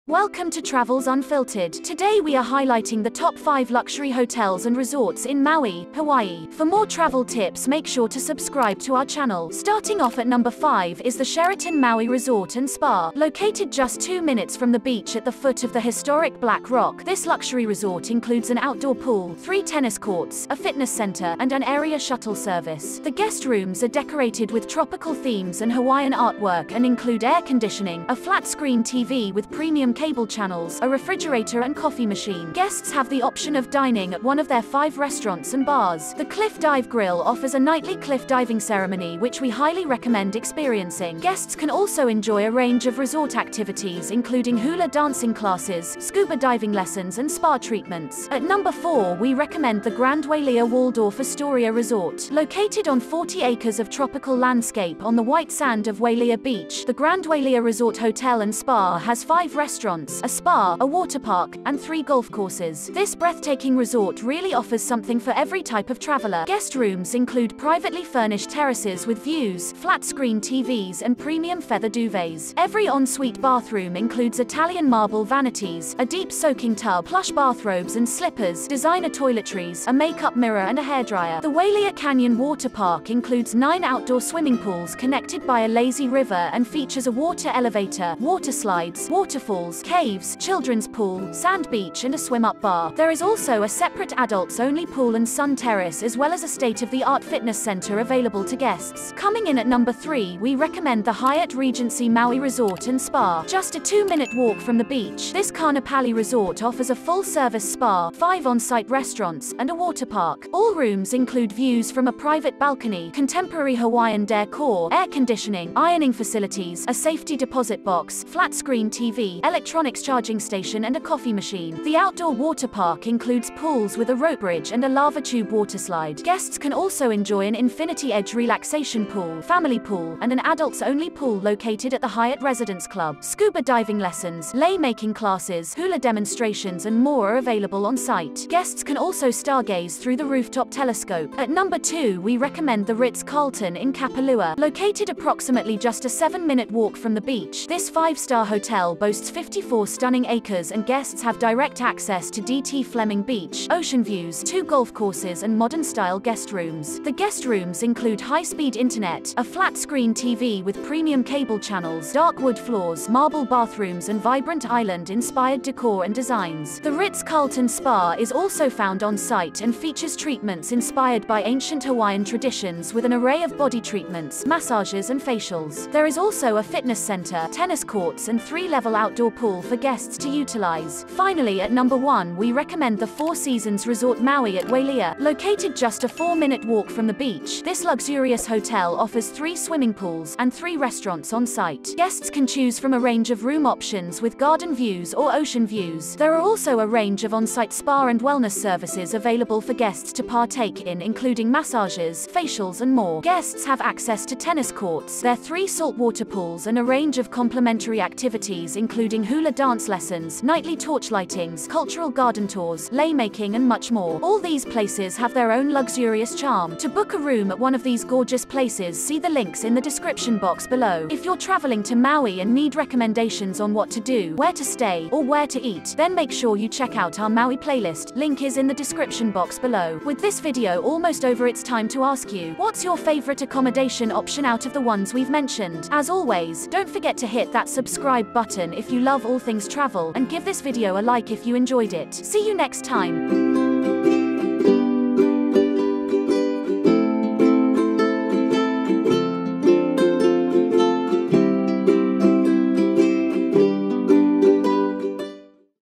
Top 5 Luxury Hotels & Resorts in Maui, Hawaii
Welcome to Travels Unfiltered. Today we are highlighting the top 5 luxury hotels and resorts in Maui, Hawaii.

Find all of our recommendations below:
(0:00): Introduction

Enjoy a $10 discount on your next restaurant booking!